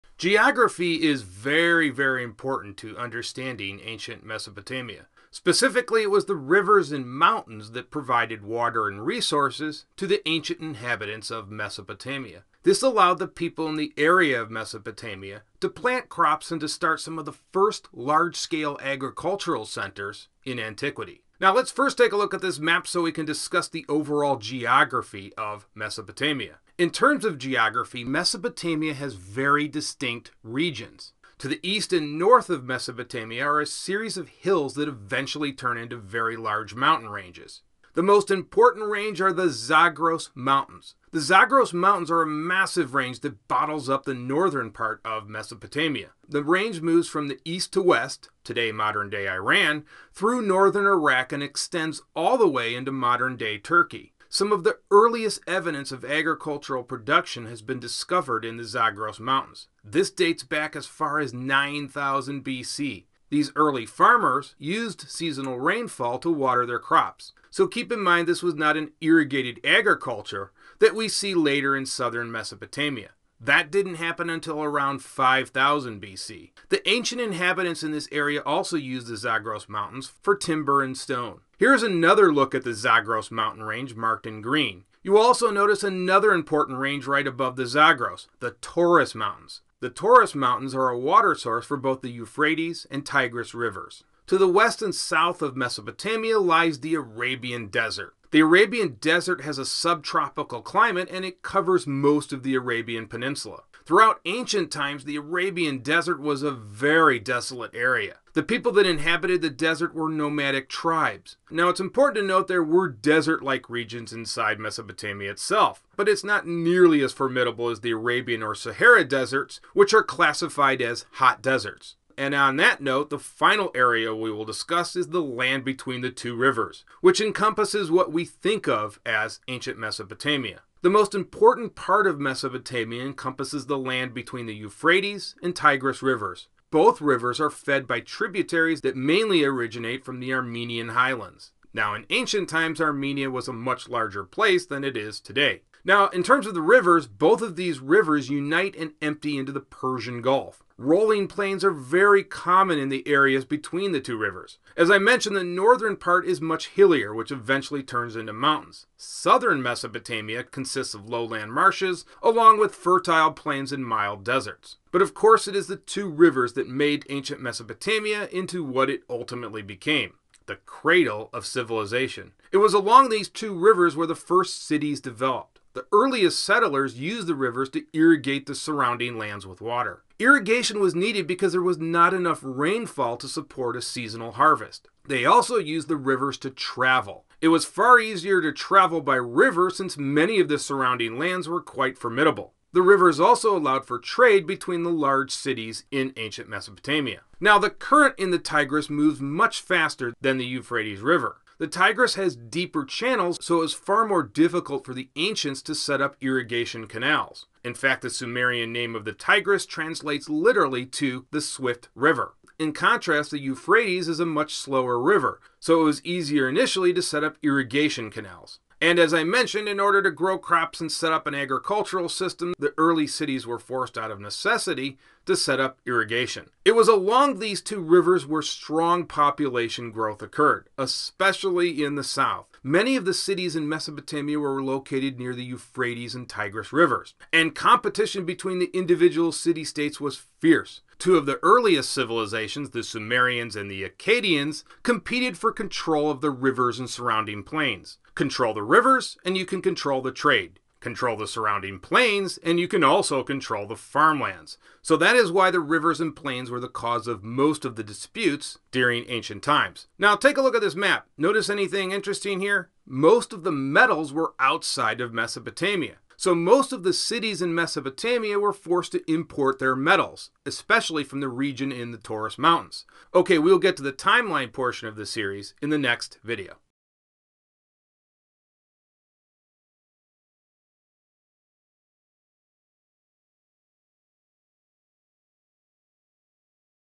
Ancient Mesopotamia - Geography - 2
In this video we will discuss geography in Ancient Mesopotamia.